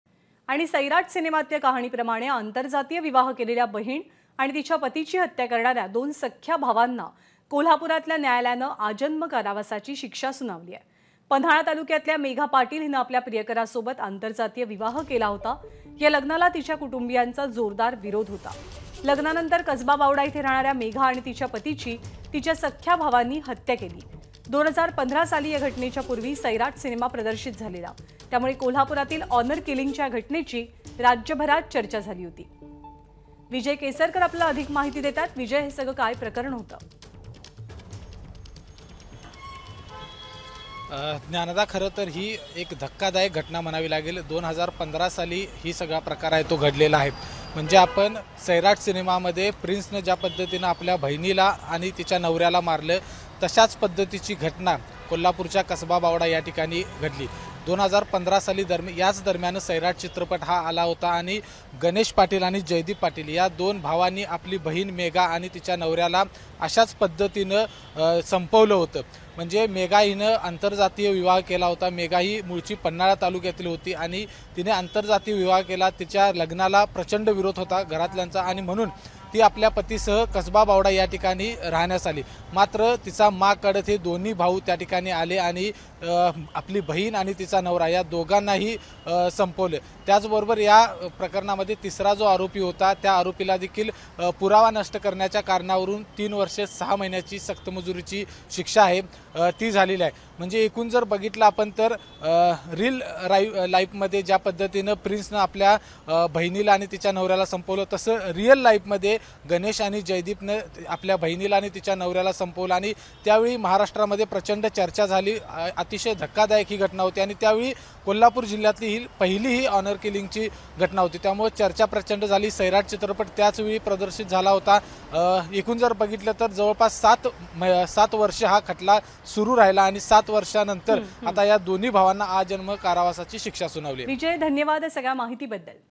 Kolhapur:आंतरजातीय विवाह केल्यानं बहिणीसह तिच्या पतीची भावांकडून हत्या, कोर्टाकडून जन्मठेपेची शिक्षा
Maharashtra Kolhapur Crime News : सैराट चित्रपटातील (Sairat Movie) कहाणीप्रमाणे आंतरजातीय विवाह केलेल्या बहीण (Sister) आणि तिच्या पतीची हत्या करणाऱ्या दोन सख्ख्या भावांना कोल्हापुरातील न्यायालयानं आजन्म कारावासाची शिक्षा (Punishment of life imprisonment) सुनावली आहे. पन्हाळा तालुक्यातल्या (Panhala Taluka) मेघा पाटील हिने प्रियकराबरोबर आंतरजातीय विवाह (Intercaste Marriage) केला होता. या लग्नाला तिच्या कुटुंबीयांचा विरोध होता. लग्नानंतर कसबा बावडा (Kasaba Bawada) इथं राहणाऱ्या मेघा आणि तिच्या पतीची तिच्या सख्ख्या भावांनी हत्या केली होती. 2015 साली या घटनेच्या आधी सैराट चित्रपट प्रदर्शित झाला होता. त्यामुळे कोल्हापुरातील ऑनर किलिंगच्या या घटनेची राज्यभर चर्चा झाली होती.